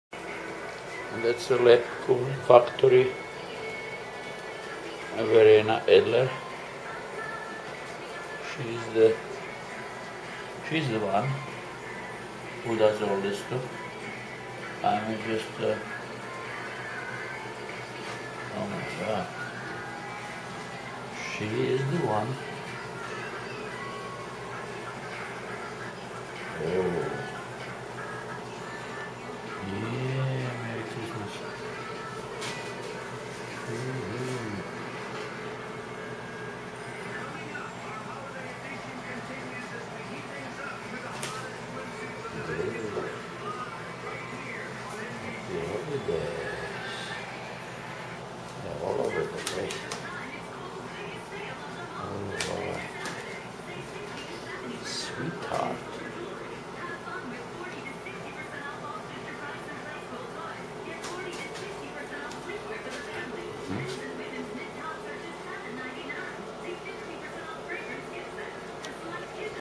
German Gingerbread Hearts
Gingerbread Hearts for Christmas! Delicious Lebkuchen Herzen hand made and decorated by Verena Edler Perfect edible one of a kind present.